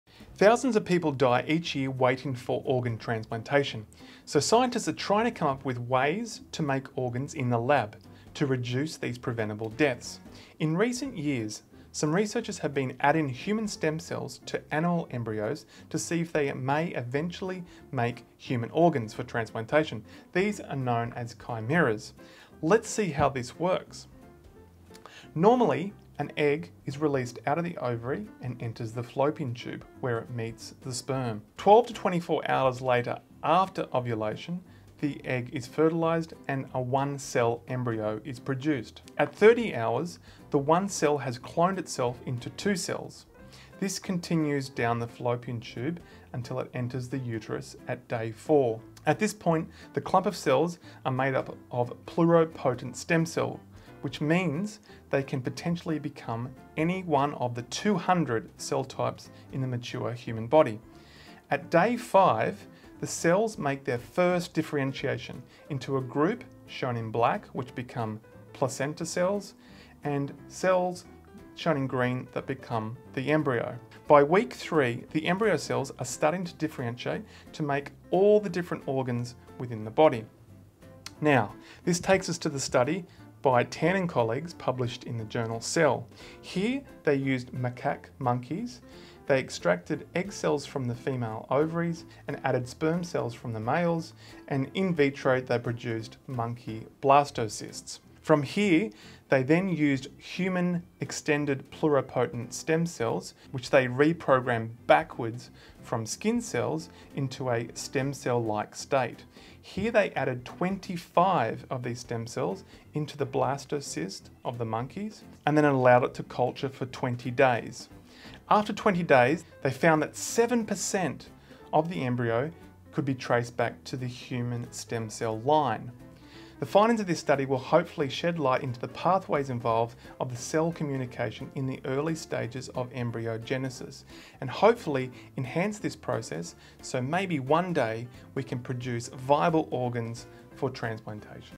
Have scientists made a monkey/human hybrid?
In this video, Dr Matt explains what a monkey/human chimera is.